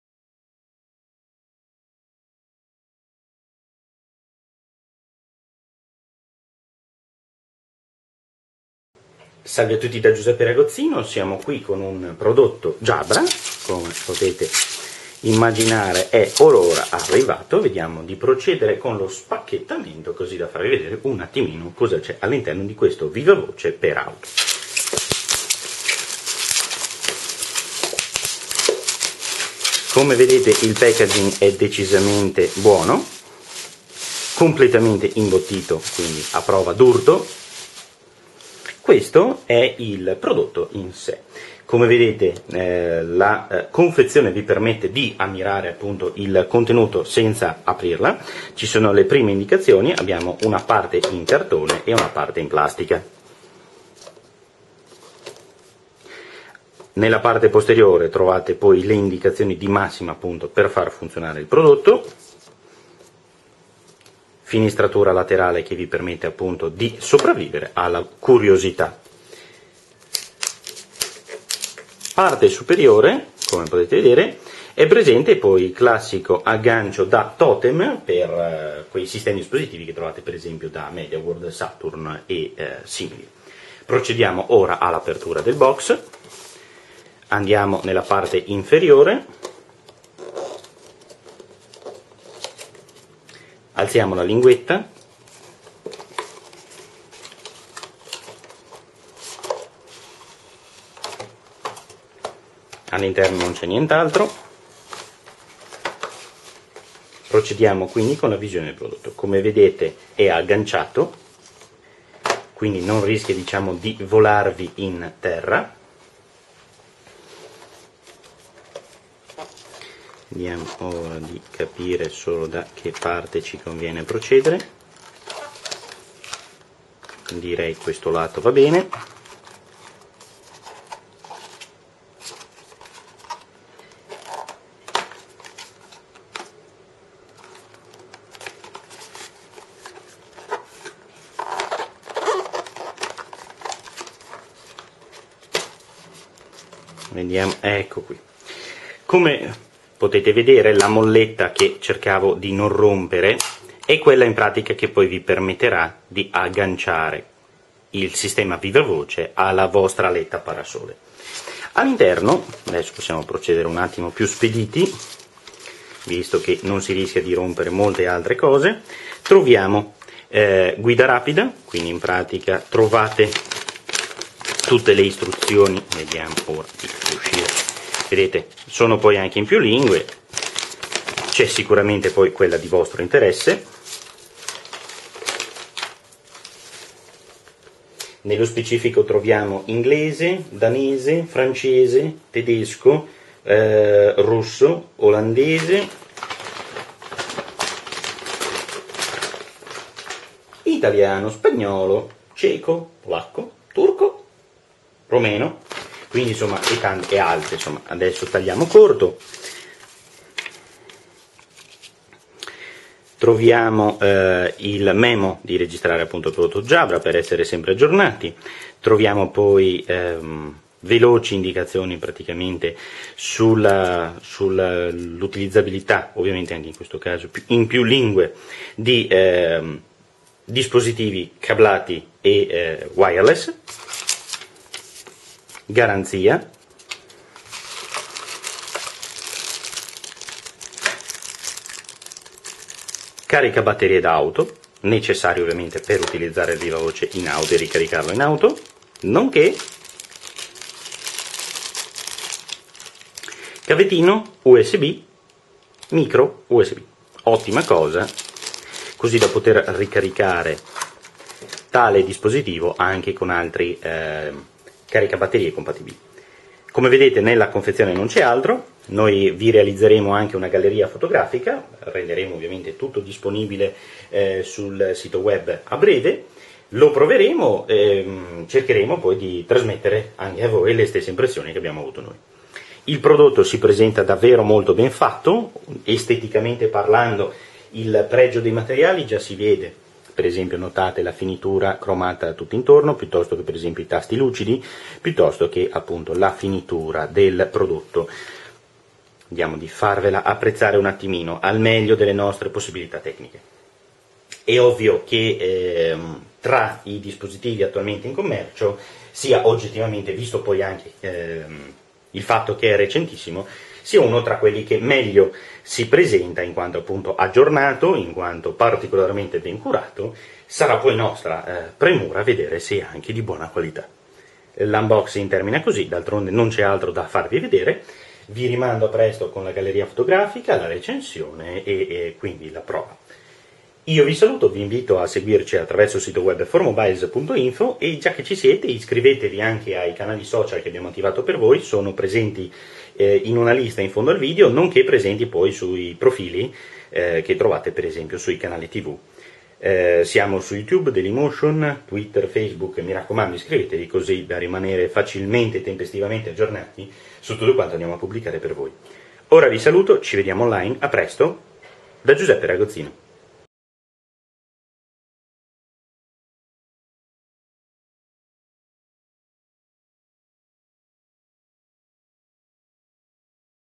Unboxing di Jabra Cruiser2 - esclusiva italiana !
Ecco a voi, in esclusiva italiana questa volta, l'unboxing del vivavoce per auto marchiato Jabra dal nome Cruiser2.
Facilmente riuscirete a capire che si tratta dell'evoluzione del modello Cruiser che ha ottenuto buona risposta da parte del pubblico, in questa variante meglio rifinita e decisamente più completa.
Esteticamente parlando abbiamo tra le mani un dispositivo nato per vivere in auto, dotato di caricabatteria adatto all'uso in strada nonchè di una comoda molletta per poter agganciare il tutto all'aletta parasole.
Sarà nostra premura provarlo, restate con noi per tutte le info a seguire.